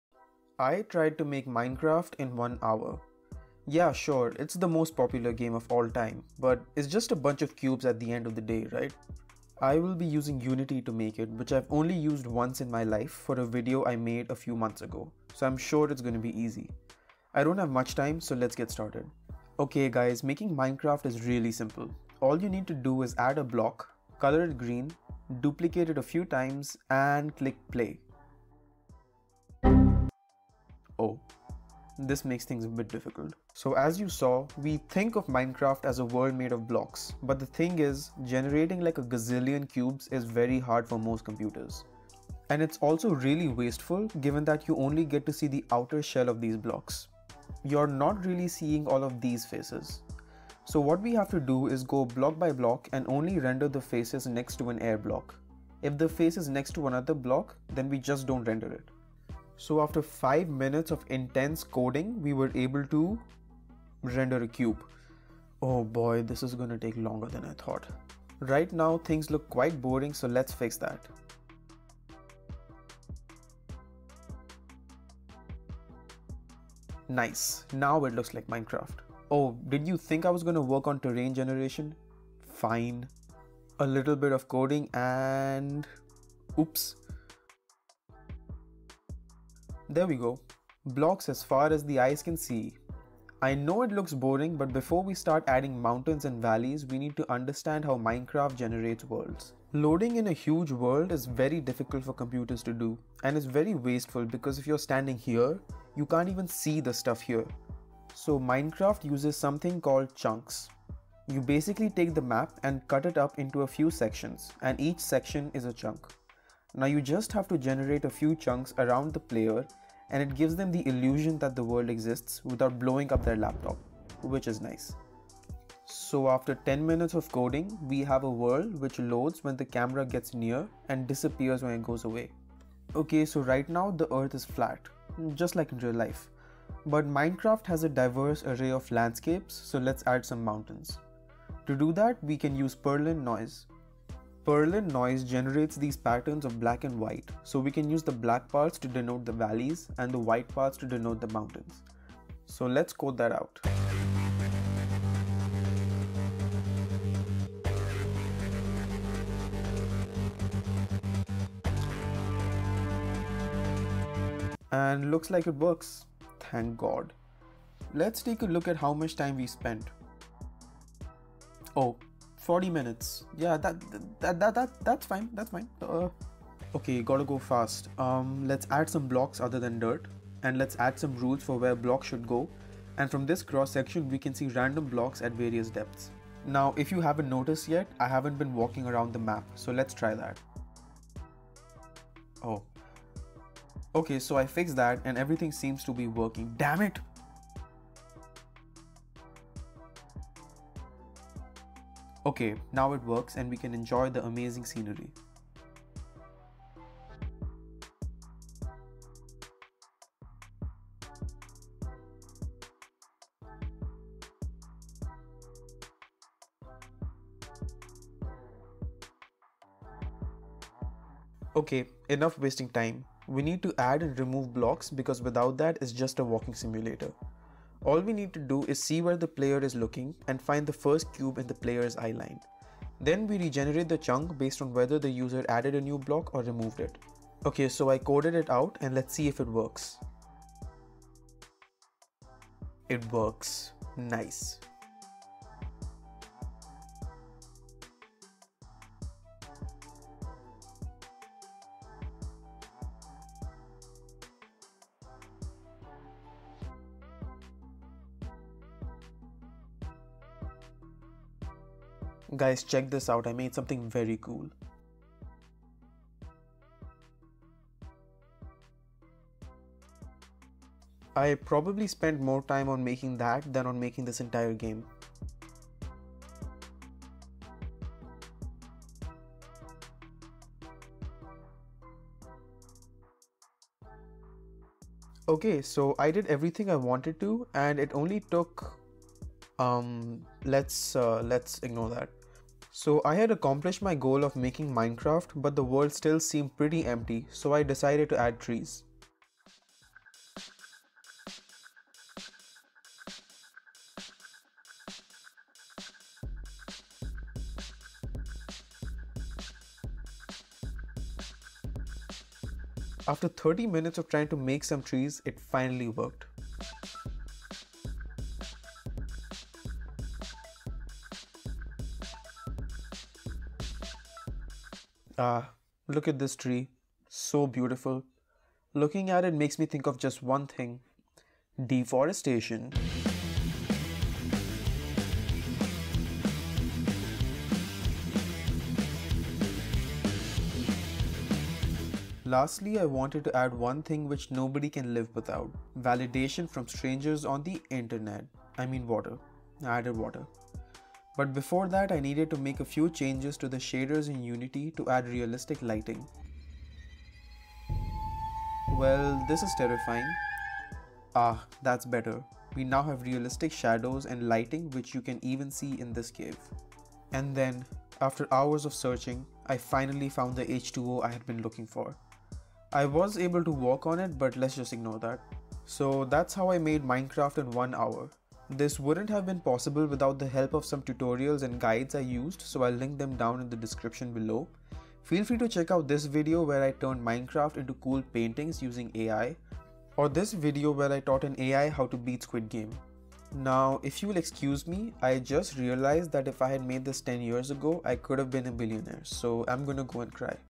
I Coded Minecraft in One Hour (Unity3D)
Is it possible to make Minecraft in just an hour using Unity3D? Open world voxel based procedurally generated games are known for being very easy to develop so I'm sure it won't be too big of a hassle. Enjoy taking a look at how I made Minecraft in one hour.

 I've seen loads of videos of people making Minecraft insanely fast and wanted to try it out so I took up the challenge of making it in Unity in 1 hour. However, I did take the help of some tutorials and guides because I'm just human man geez:

Chapters:
00:00-00:20 Introduction
00:20-01:17 Making voxels so my laptop doesn't crash
01:17-02:12 Terrain generation (chunks)
02:12-02:47 Terrain generation (mountains)
02:47-03:06 Adding more block types
03:06-03:40 Player controller and moving around
03:40-04:52 Adding and removing blocks
04:52-05:41 Trees!
05:41-06:15 Water???
06:15-06:40 Conclusion